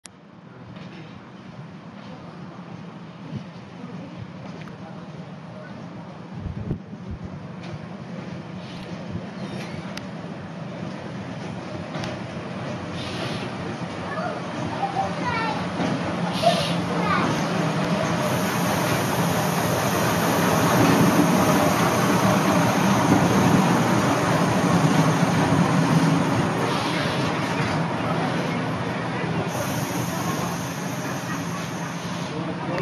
1953 railcar featured in 1983 railway magazine
small interesting little railcar with good viewing for passengers on poppy line